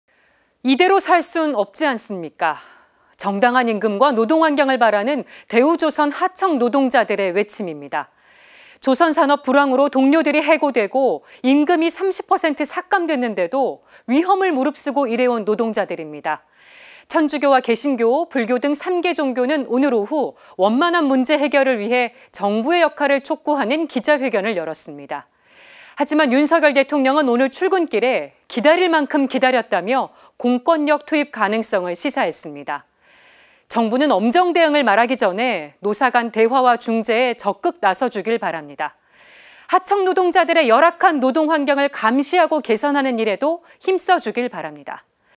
클로징 | "대우조선 하청노동자들의 절박한 외침"
"이대로 살 순 없지 않습니까?"

정당한 임금과 노동 환경을 바라는
대우조선 하청노동자들의 외침입니다.

조선 산업 불황으로 동료들이 해고되고,
임금이 30% 삭감됐는데도
위험을 무릅쓰고 일해온 노동자들입니다.

천주교와 개신교, 불교 등 3개 종교는
오늘 오후 원만한 문제 해결을 위해
정부의 역할을 촉구하는 기자회견을 열었습니다.

하지만 윤석열 대통령은 오늘 출근길에
"기다릴 만큼 기다렸다"며
공권력 투입 가능성을 시사했습니다.

정부는 엄정대응을 말하기 전에
노사간 대화와 중재에 적극 나서주길 바랍니다.

하청노동자들의 열악한 노동환경을
감시하고 개선하는 일에도 힘써주길 바랍니다.

이상으로 뉴스를 마칩니다.
시청해주신 여러분 고맙습니다.

후원문의, 매일미사 지향 신청 : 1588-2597
카카오톡 플러스친구 : cpbc TV